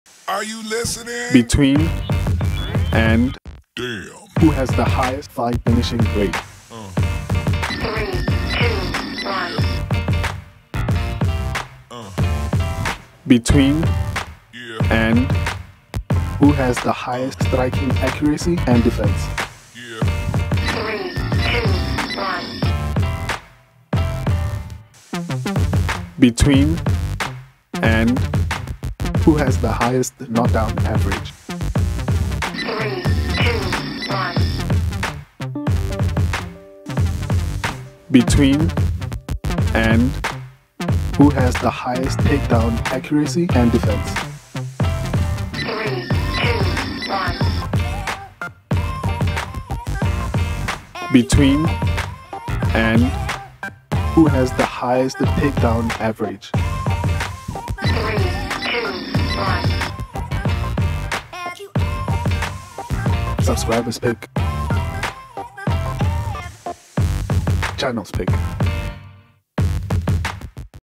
UFC Picks & Stats Quiz: Jailton Almeida vs Derrick Lewis | Fight Breakdown | EP 103
Dive into our Picks / Predictions & Fight Statistics for Jailton Almeida vs Derrick Lewis for UFC Fight Night! Join the Trivia fun in Episode 103. Who's got the edge? Tune in!!